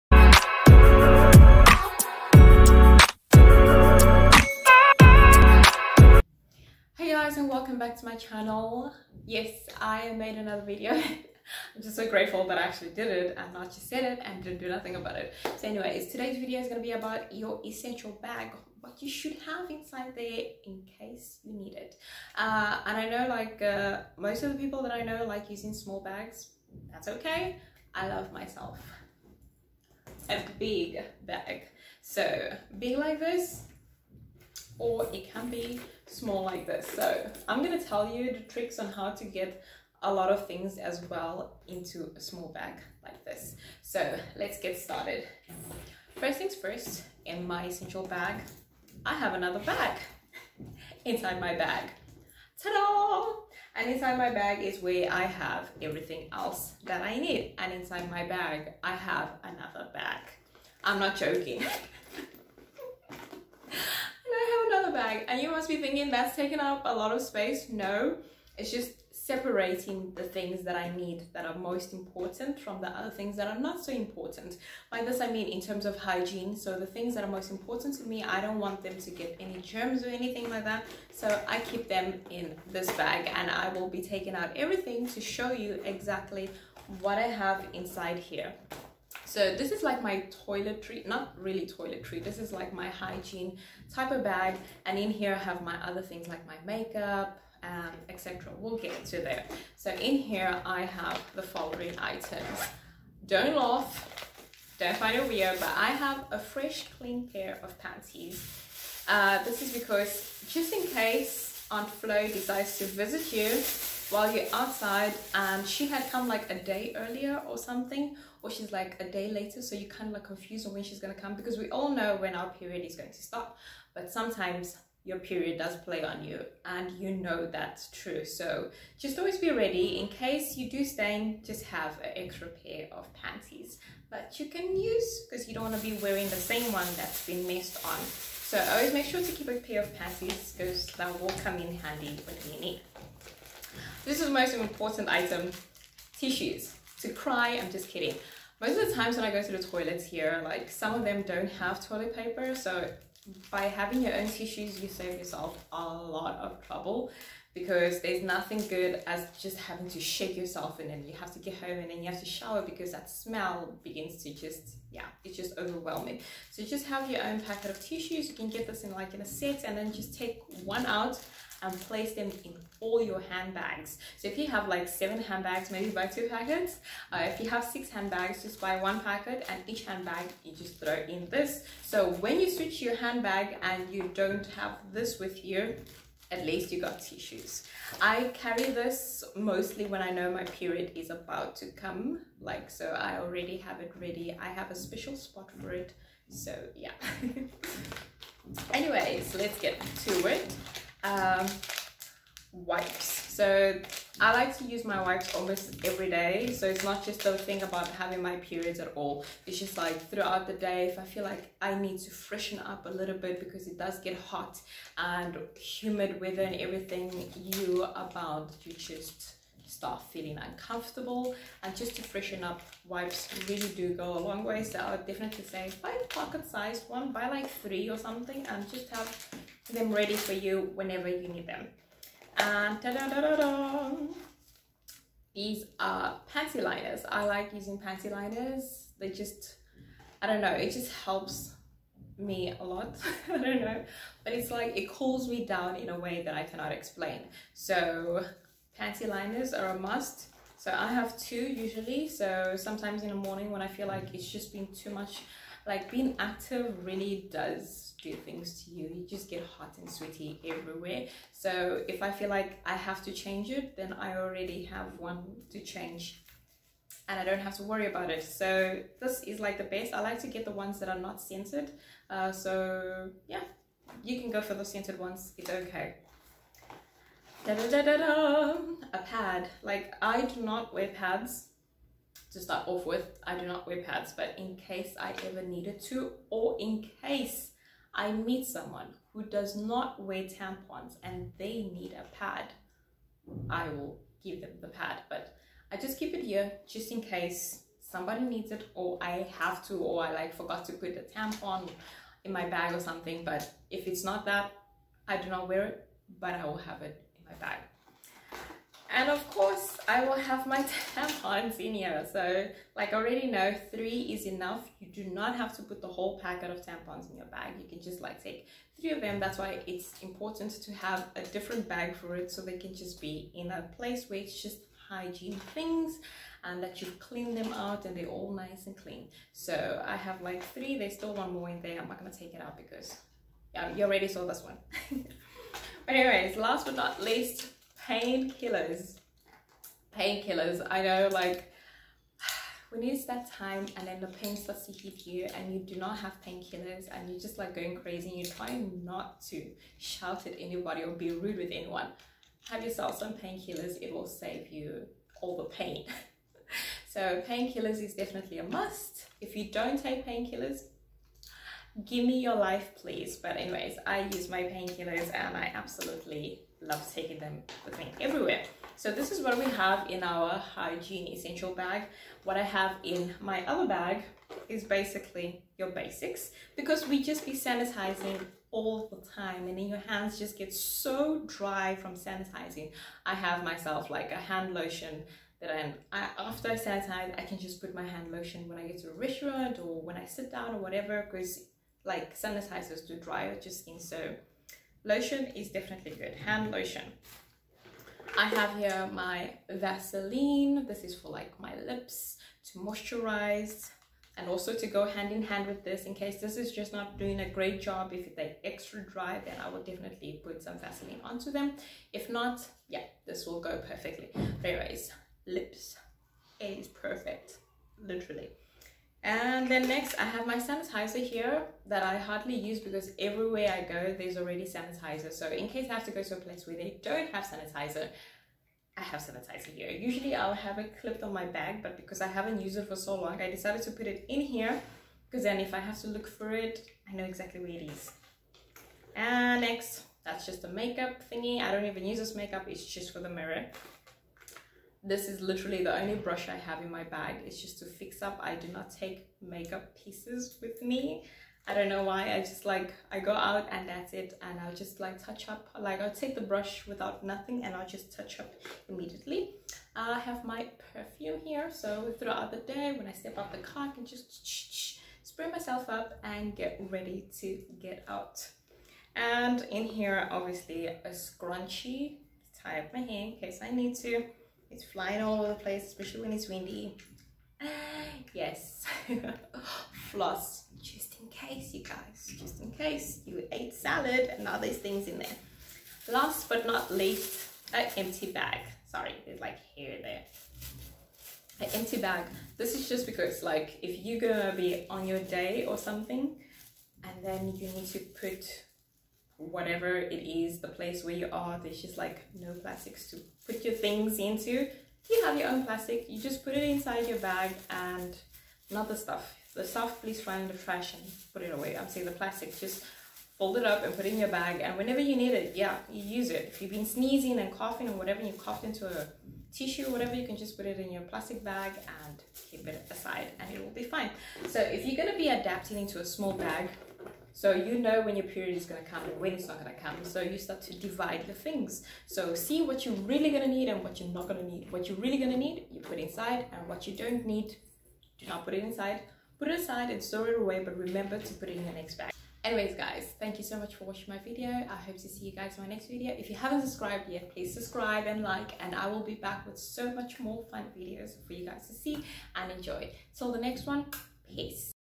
Hygiene Bag Essentials | A Must Have
Hey guys. In this Video I'm showing what items to carry when aunt Flow is about to make a visit.